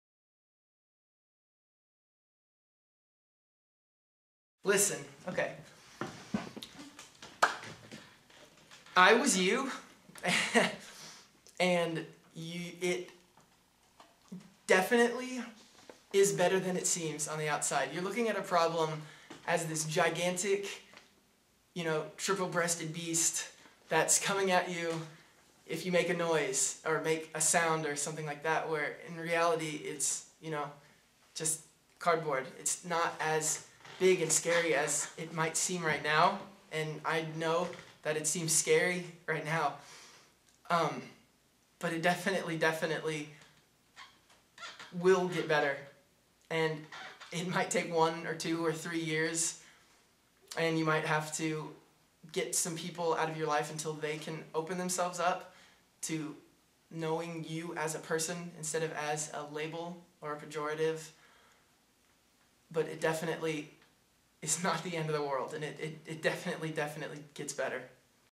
It Gets Better - RENT cast member JC Schmidt
A message from RENT cast member JC Schmidt